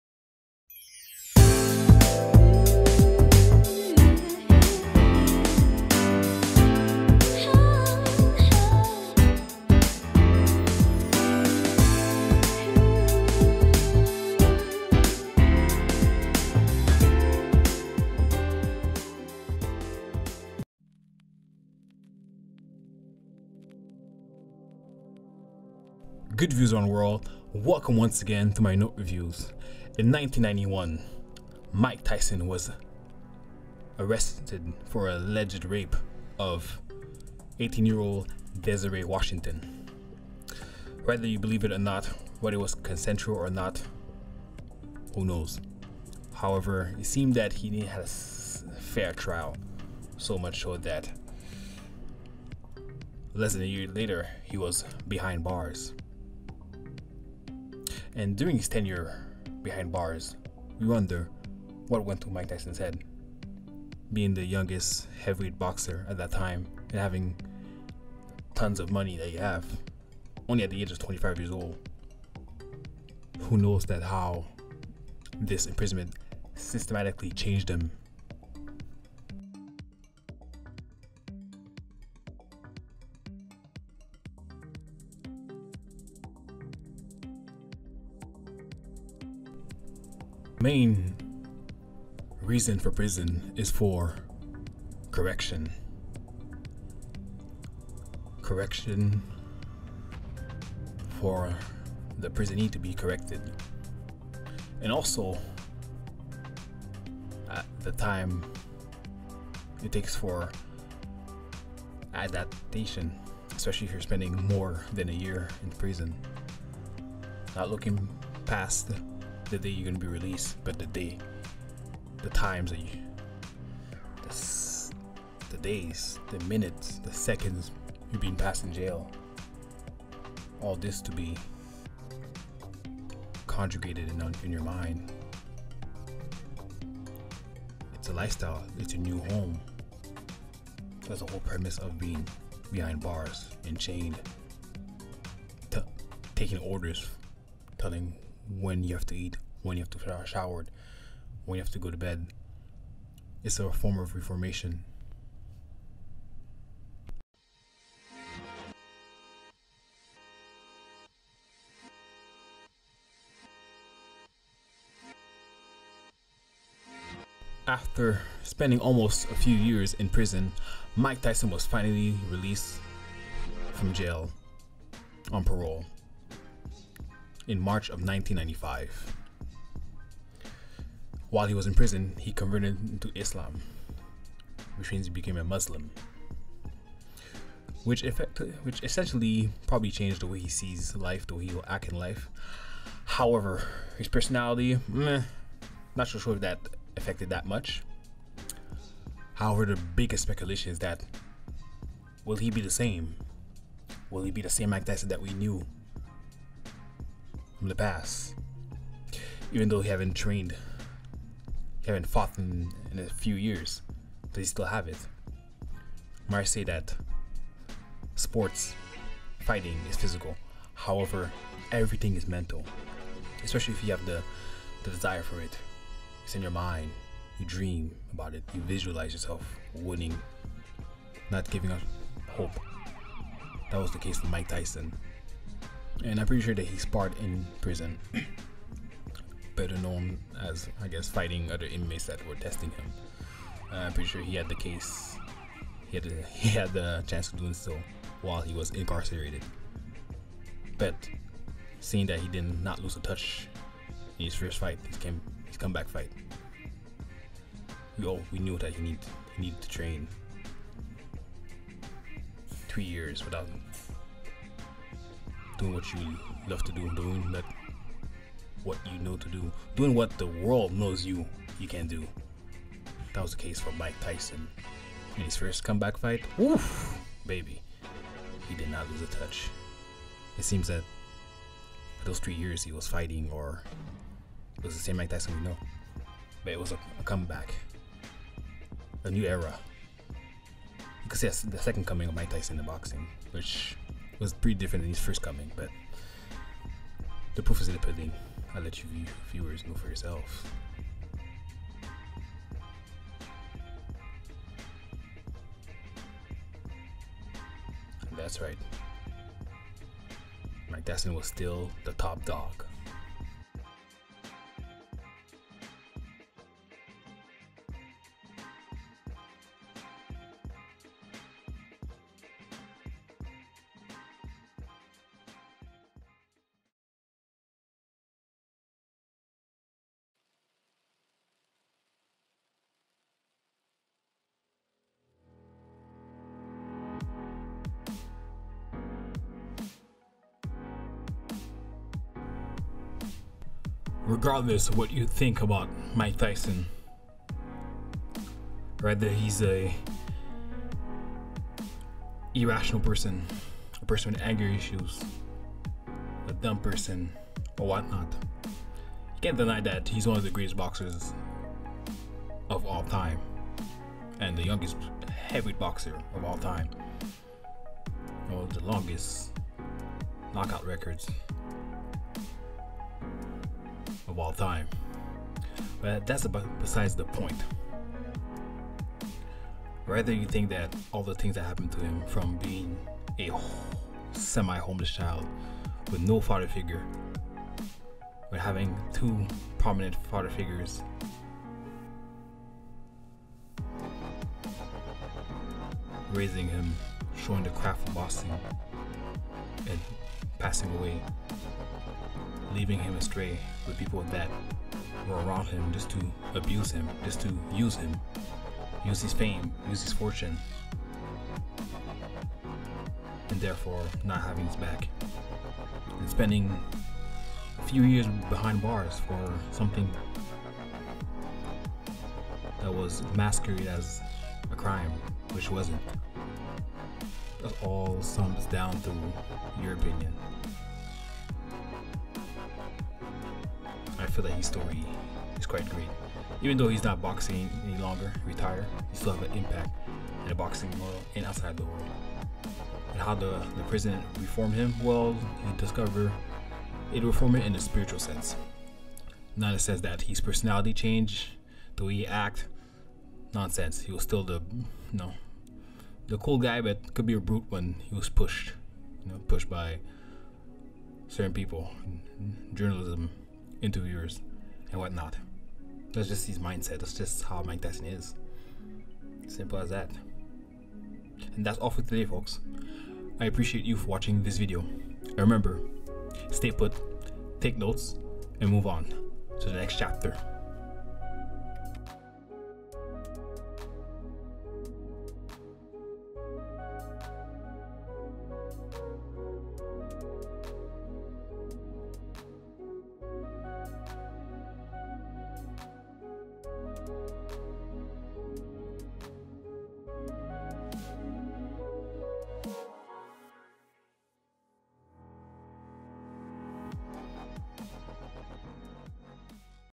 Pragmatic of Reformation - Mike Tyson
Mike Tyson was sentenced to six years in prison for the alleged rape of Desiree Washington. he was released in March 1995 after serving less than three years of his six-year sentence.During his incarceration, Mike Tyson converted to Islam. He took the Muslim name Malik Abdul Aziz.